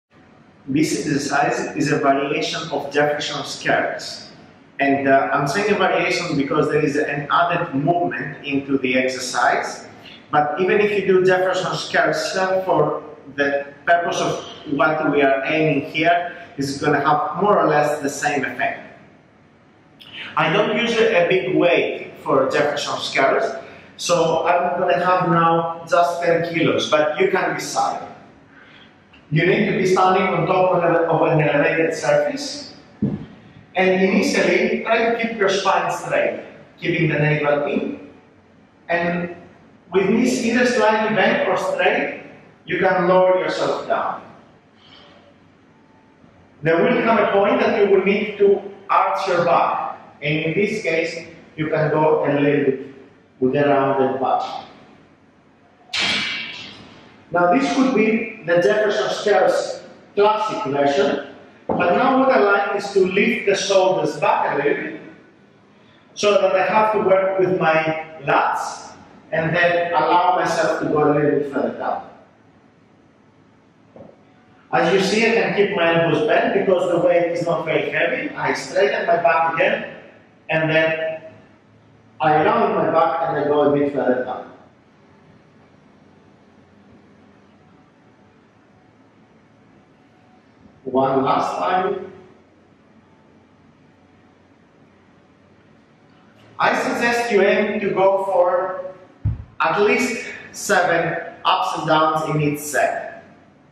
Jefferson's Curls (depressed shoulderblades variation)
Jefferson Curls is an excellent exercise for increasing flexibility in the hamstrings! It is very popular among gymnast's as well as Crossfit athletes.

Aside hamstrings Jefferson Curls can help with flexibility in the entire back of the body (superficial back line). In its traditional version the practitioner allows the weight to fold his body in half. In the variation described here, you can promote flexibility both with your back straight & rounded. When the back rounds in most cases the upper back is stretched but not the lower back (unless you are already flexible enough with little limitations in hips and hamstrings).

When the shoulder-blades are depressed the spine is straight (or straighter) and when forward bending you should feel an additional stretch in your lower back compared to the classical version.